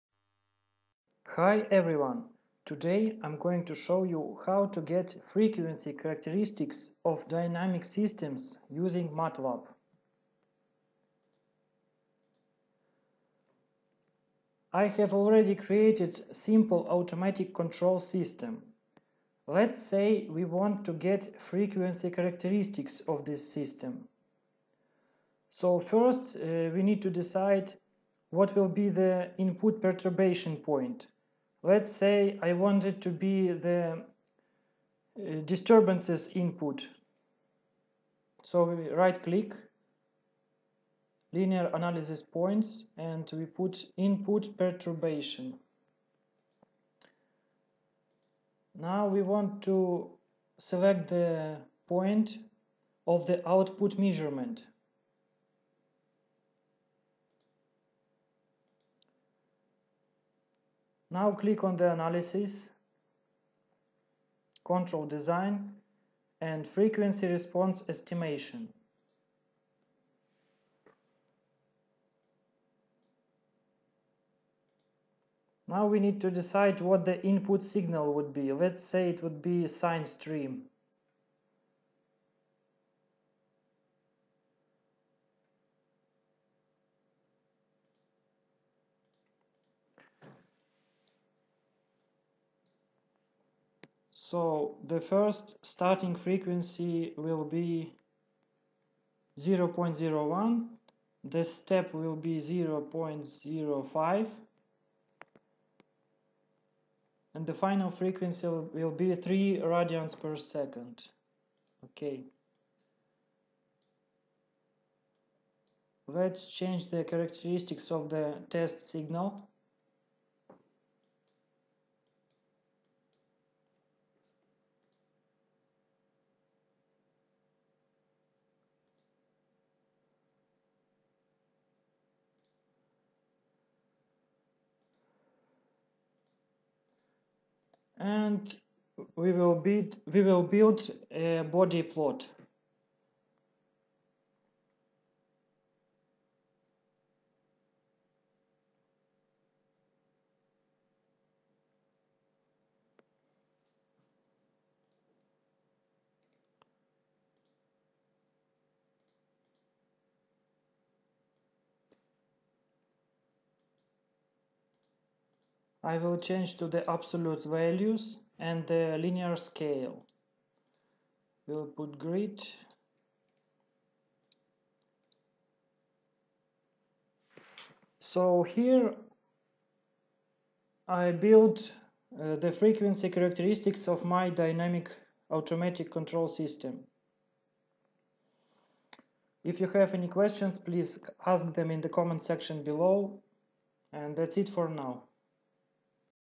MatLab Simulink frequency characteristics
In this video I'm showing how to build frequency characteristics of dynamic system using matlab simulink.